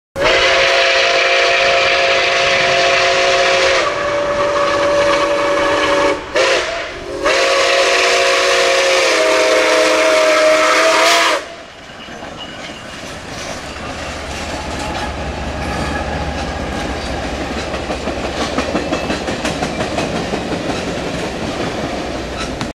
NKP 765 Test Run in New Haven
I went with my friend Derek to go see NKP 765 on a test run on Norfolk Southern trackage. It was running from New Haven to Muncie IN and back. Sadly we only caught the Train here in New Haven, IN.